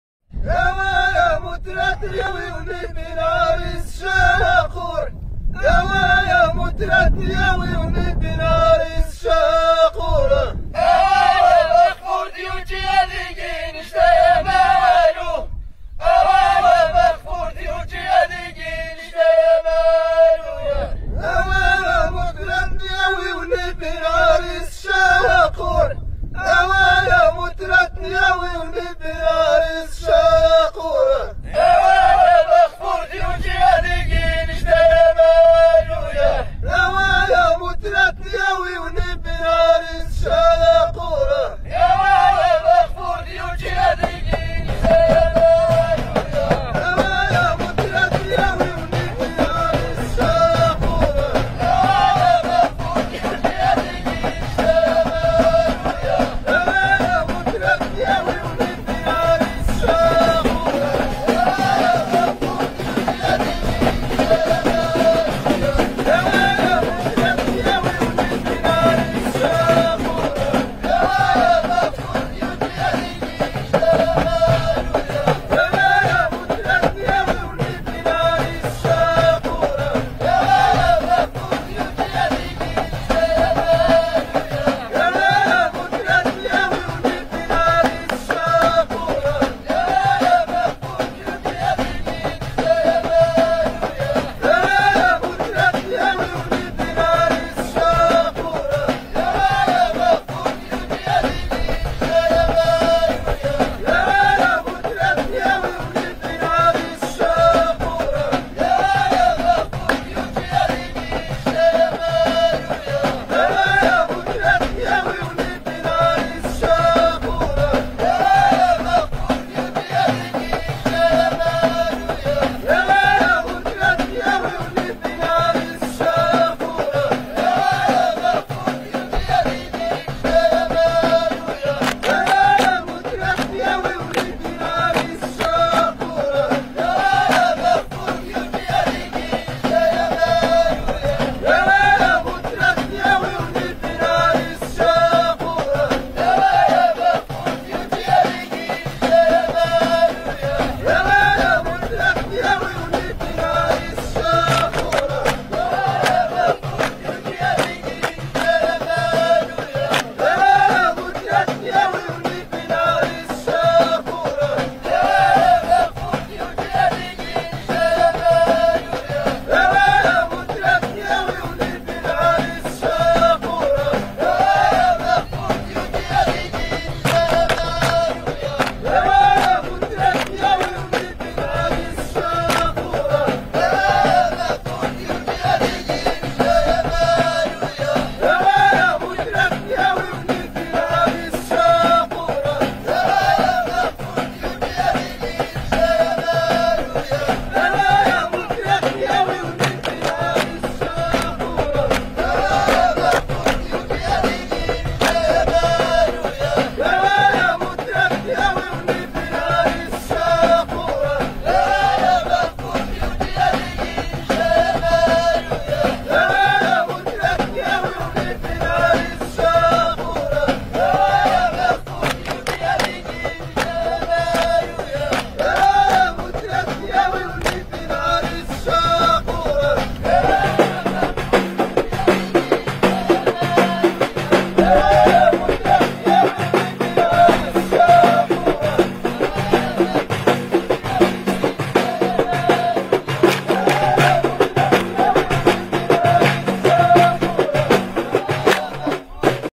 اخف واسيف . (تمركلت) أحيدوس أمازيغي
جمعية اخف واسيف . (تمركلت) أحيدوس أمازيغي أصيل .لكنه مختلف عن الاخرين . زيادة في  الحركات وتتاسق كبير .ما رأيكم